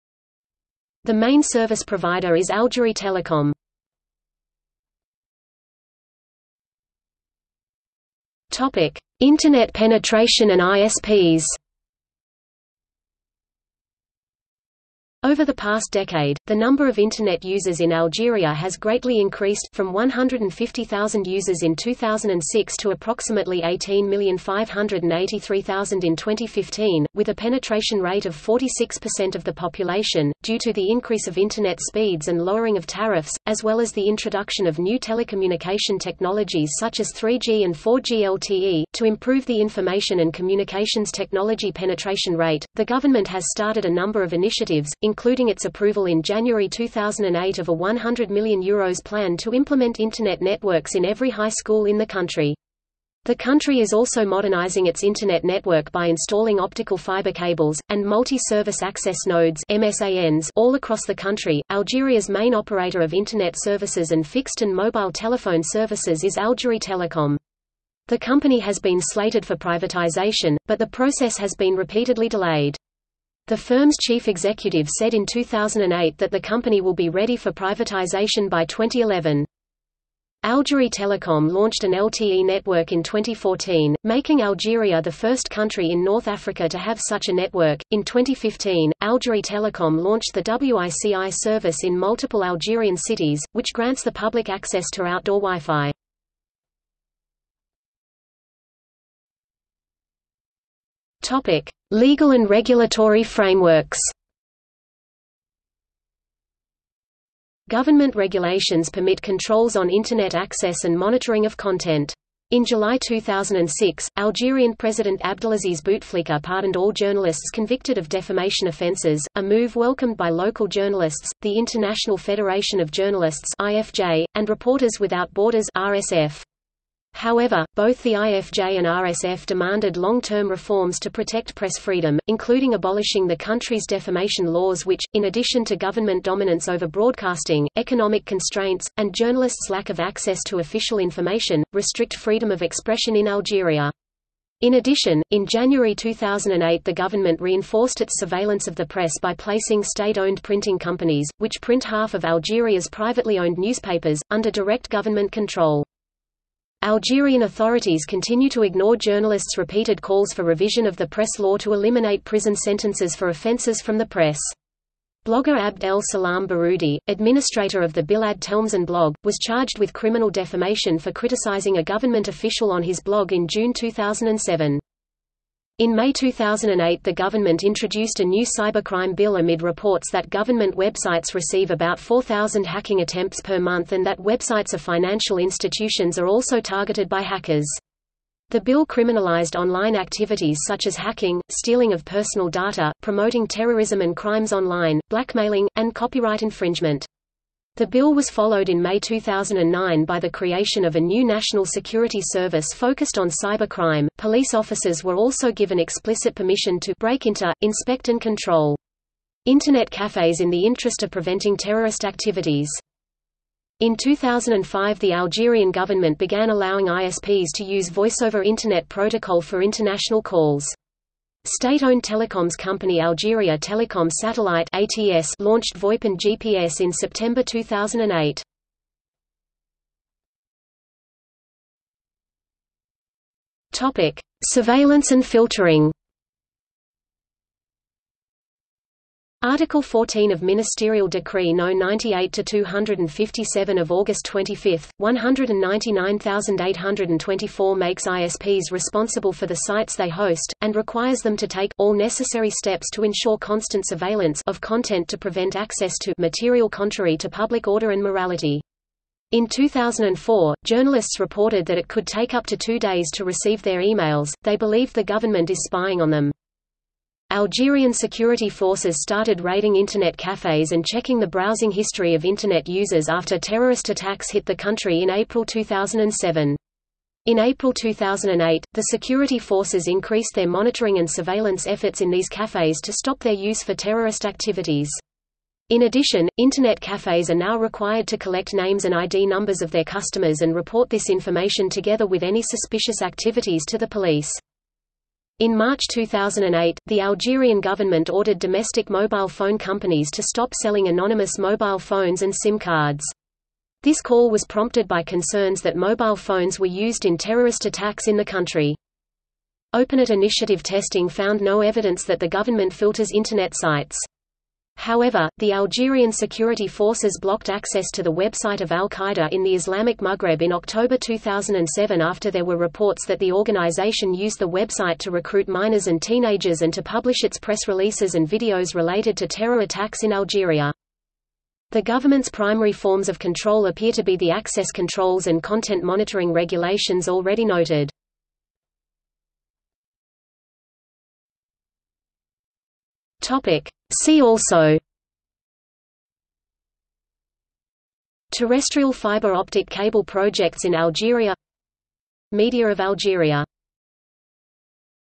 Internet in Algeria | Wikipedia audio article
00:00:05 1 Internet penetration and ISPs
00:01:37 2 Legal and regulatory frameworks
00:03:50 3 Surveillance and filtering
00:05:39 4 See also

- Socrates

SUMMARY
The main service provider is Algérie Télécom.